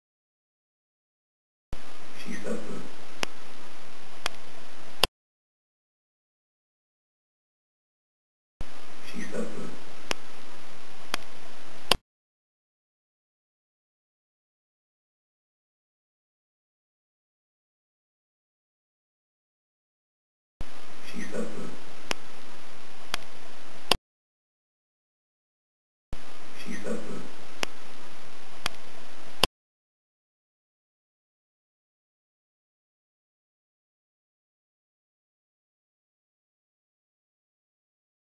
'She Suffered'
The clip pretty much speaks for itself. I suggest you have the volume up full and as it is only about three seconds in length and despite me putting on four times; you may have to playback to hear it more clearly. I had to play it back two or three times; as I couldn't believe what I had heard. Recorded between June through to August 2006. Unsure of exact date.